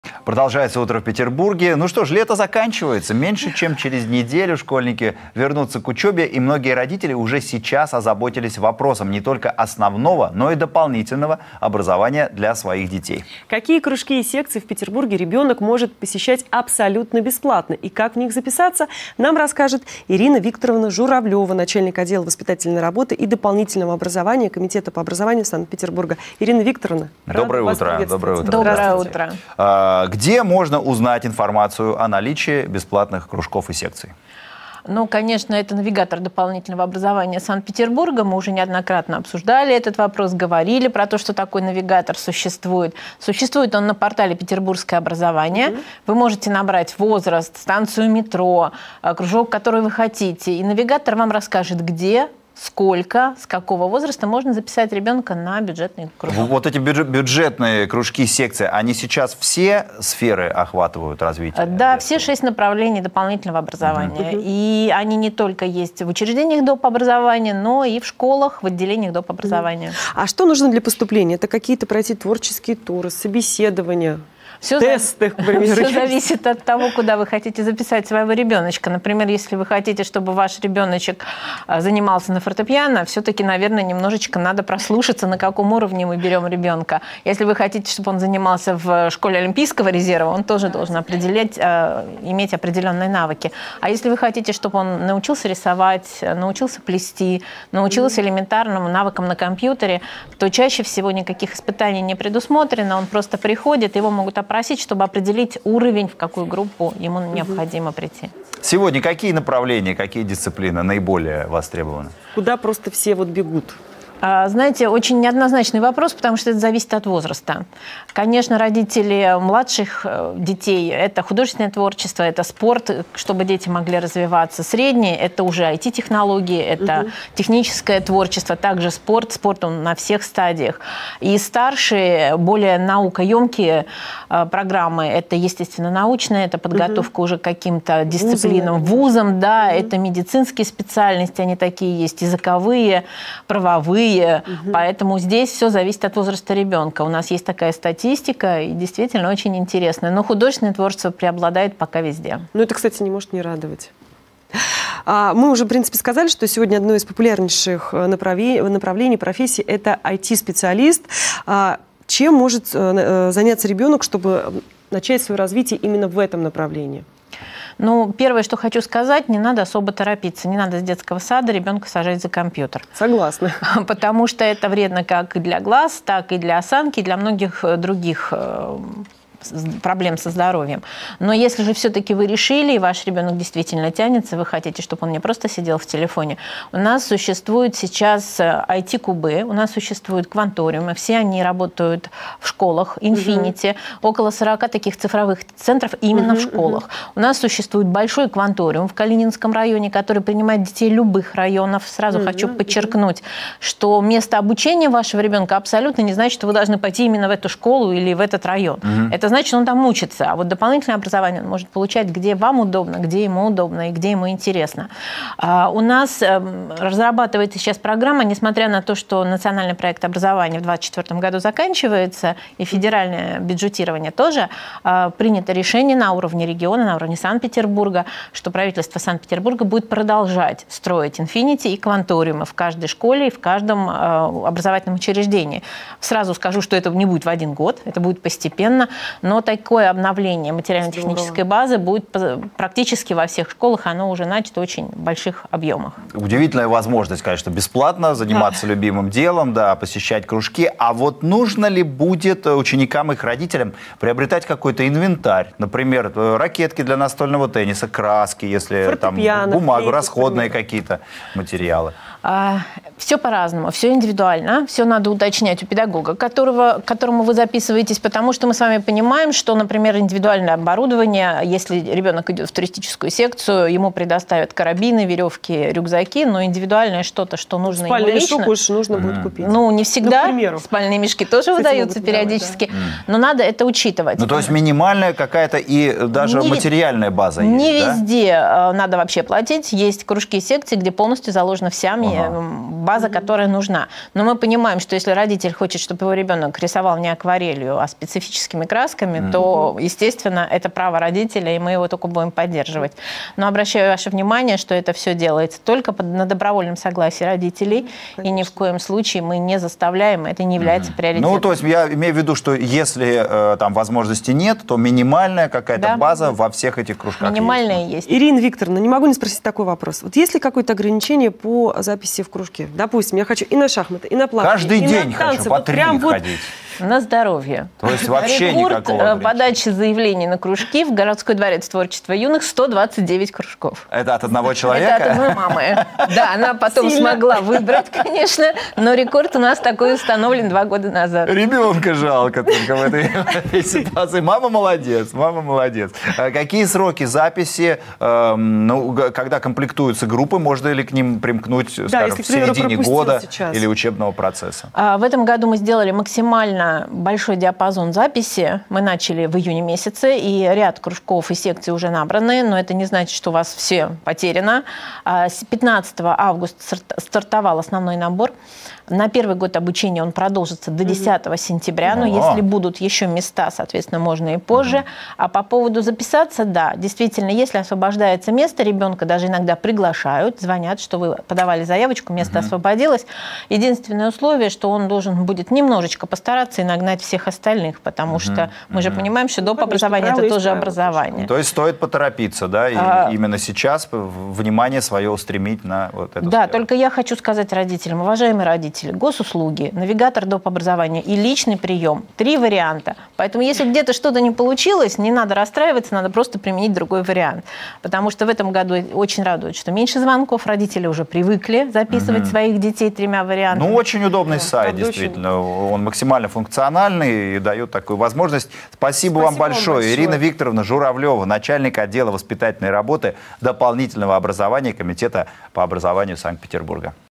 Какие кружки и секции в Петербурге ребёнок может посещать бесплатно
В студии программы «Утро в Петербурге» Ирина Журавлёва, начальник Отдела воспитательной работы и дополнительного образования Комитета по образованию Санкт-Петербурга.